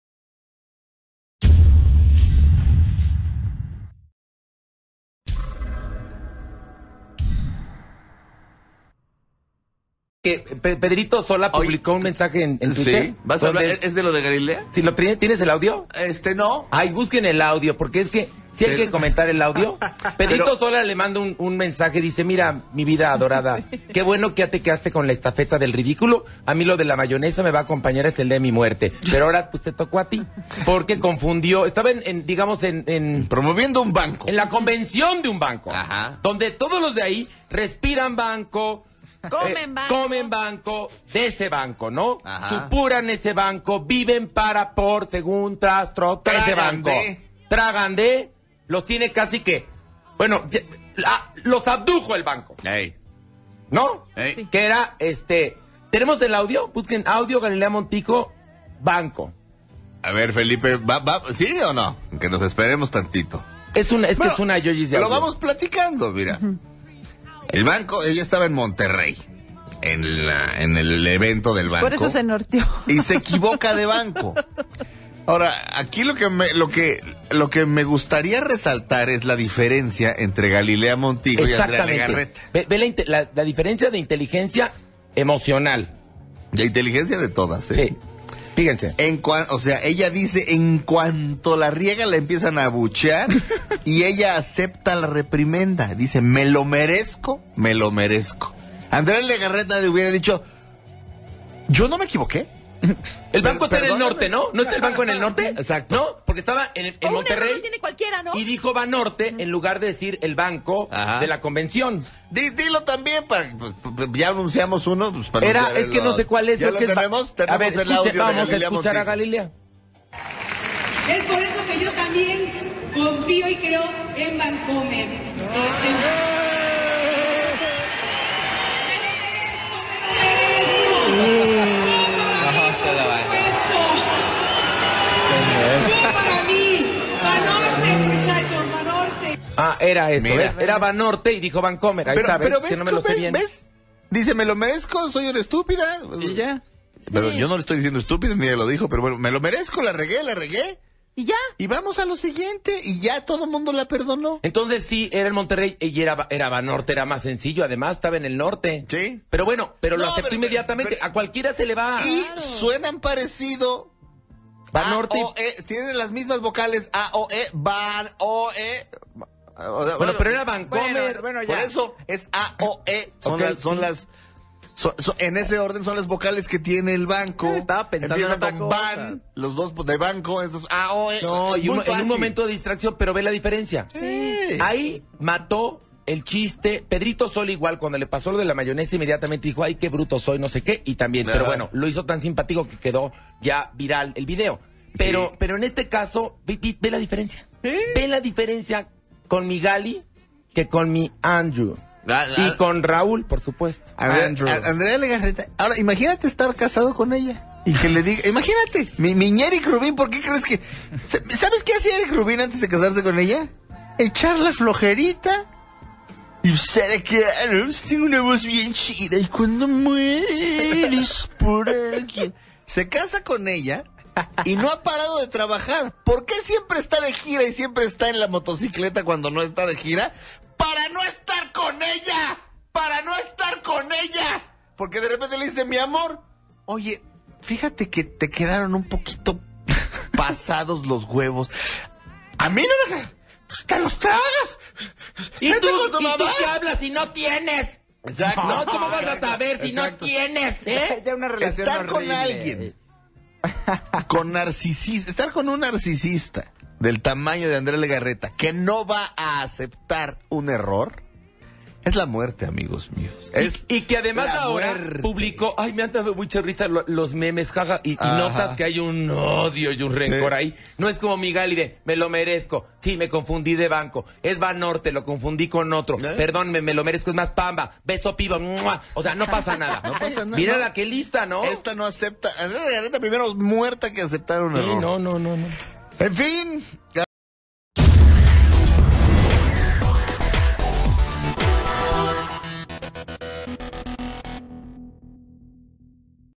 Abuchean a Galilea Montijo por Confundir Nombre de Banco
Un banco contrató a Galilea Montijo para que condujera su evento, pero la conductora perdió la concentración mientras convivía con el público.

La presentadora la traicionó el inconsciente, y cuando tenía que mencionar el nombre de la empresa que la llevó a Monterrey, dijo el nombre de un banco de la competencia.

El momento quedó grabado en un video donde se escucha a la gente abuchearla, y a ella aceptar la reprimenda: "¡Me lo merezco, me lo merezco!".